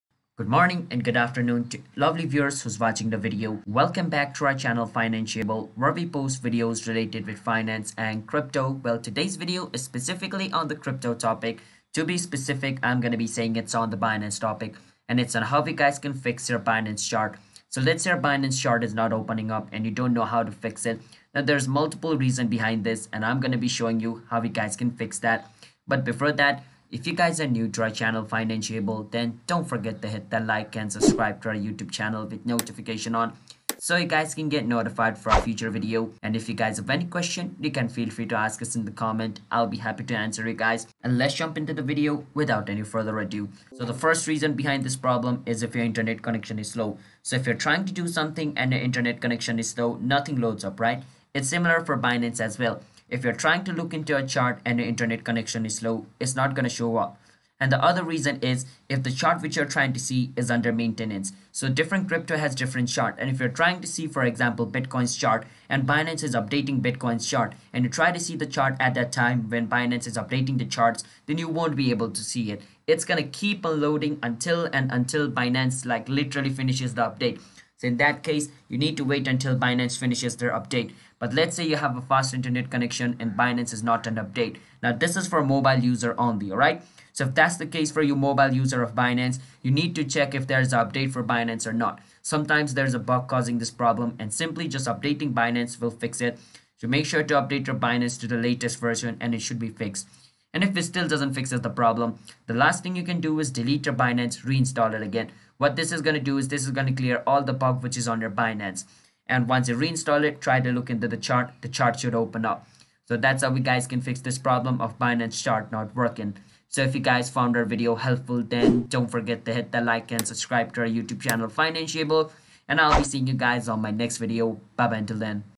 How To Fix Binance Chart | Binance TradingView Charts Not Working [SOLVED]
Learn the exact steps on How To Fix Binance Chart. Make sure you watch the video till the end for better assistance.

CHAPTERS
0:00 - Intro
0:17 - How To Fix Binance Chart
1:53 - Outro